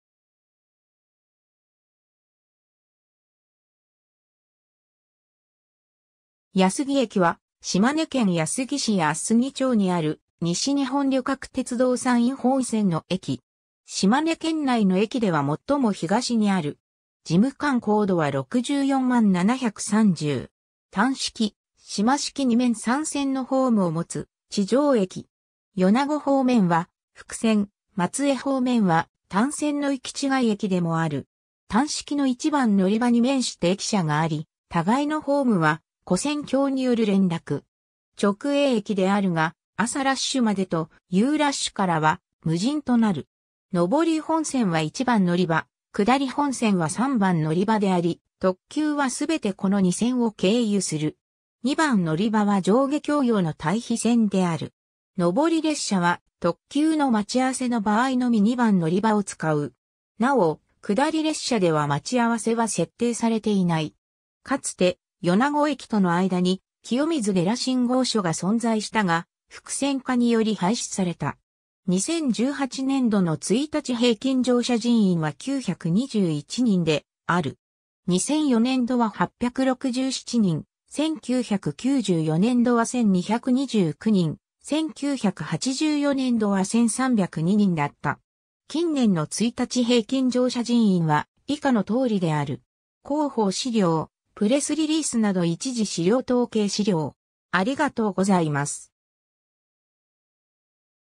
安来駅
安来駅（やすぎえき）は、島根県安来市安来町にある、西日本旅客鉄道（JR西日本）山陰本線の駅。
島根県内の駅では最も東にある。
 単式・島式2面3線のホームを持つ地上駅。
米子方面は複線（伯耆大山駅まで）・松江方面は単線の行違い駅でもある。
単式の1番のりばに面して駅舎があり、互いのホームは跨線橋による連絡。
直営駅（松江駅の被管理駅）であるが、朝ラッシュまでと夕ラッシュからは無人となる。
(係員対応時間8:45~17:00) 上り本線は1番のりば、下り本線は3番のりばであり、特急は全てこの2線を経由する。
 2番のりばは上下共用の待避線（中線）である。
上り列車は特急の待ち合わせの場合のみ2番のりばを使う。
なお、下り列車では待ち合わせは設定されていない。
 かつて米子駅との間に、清水寺信号所が存在したが、複線化により廃止された。
 2018年（平成30年）度の1日平均乗車人員は921人である。
2004年度は867人、1994年度は1,229人、1984年度は1,302人だった。
 広報資料・プレスリリースなど一次資料 統計資料